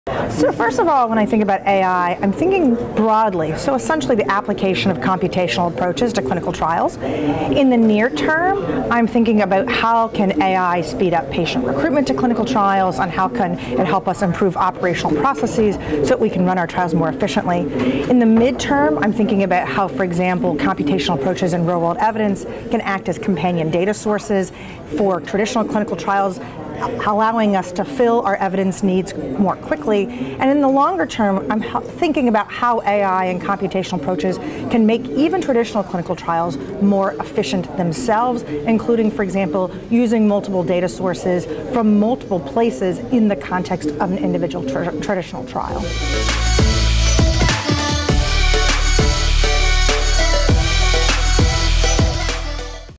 Amy Abernethy on ways AI can improve clinical trials
Amy Abernethy, MD, PhD, Deputy Commissioner, FDA, discusses the implications of AI-enabled tools for clinical trial design and management at the 2019 World Medical Innovation Forum.